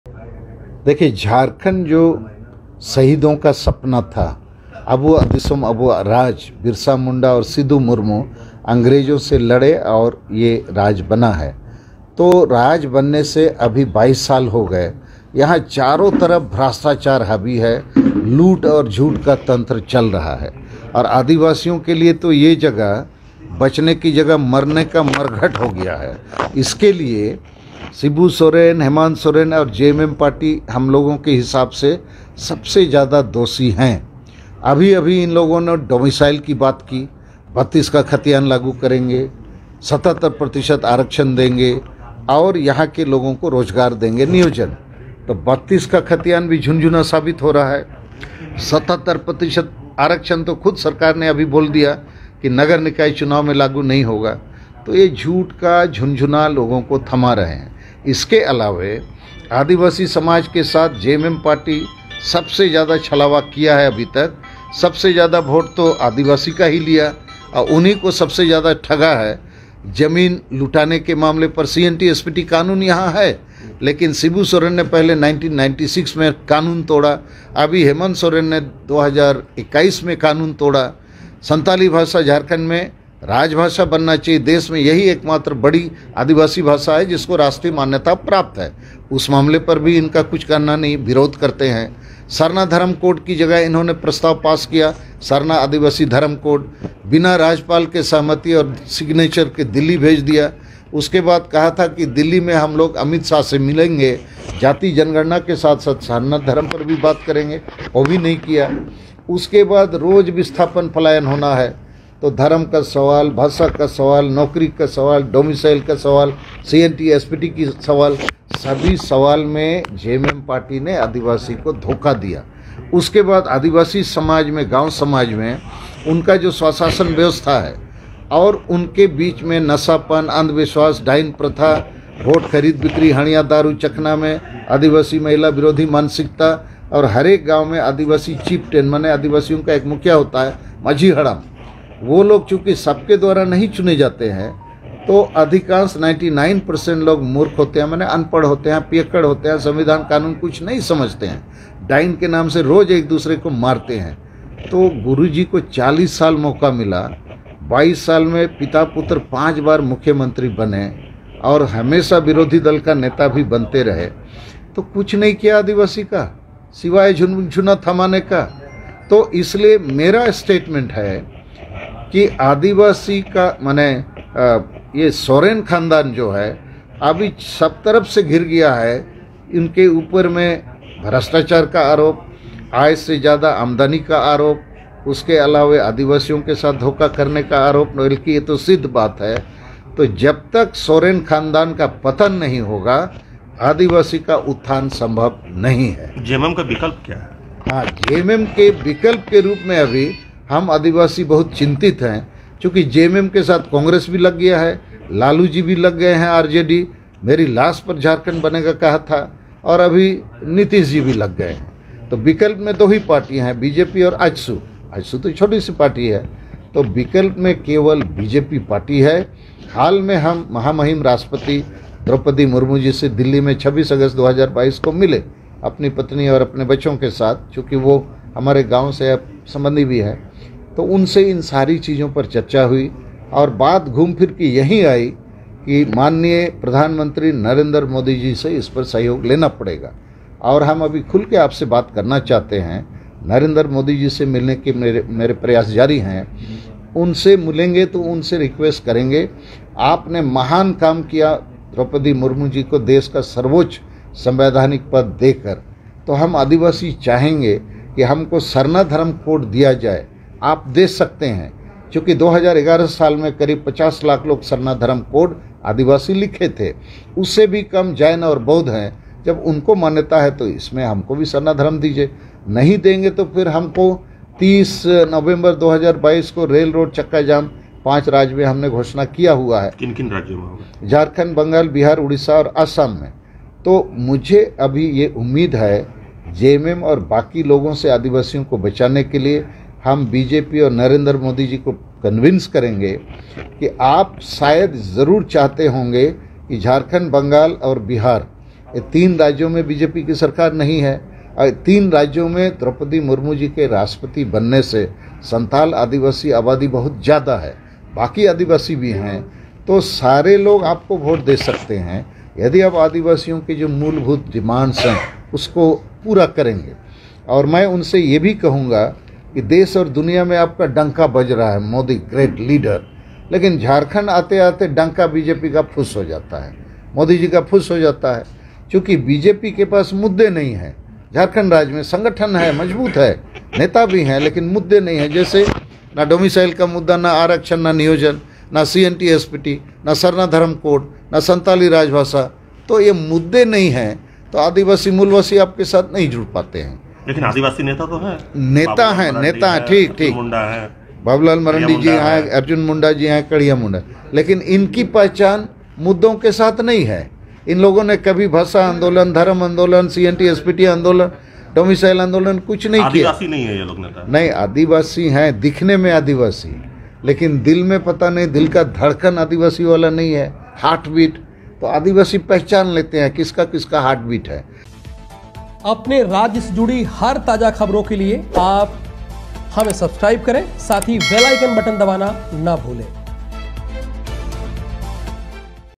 JMM पार्टी आदिवासियों के साथ सबसे ज्यादा छलावा किया है- सलखान मुर्मू
Salkhan Murmu
hemant soren
shibu soren
bjp
jmm
rjd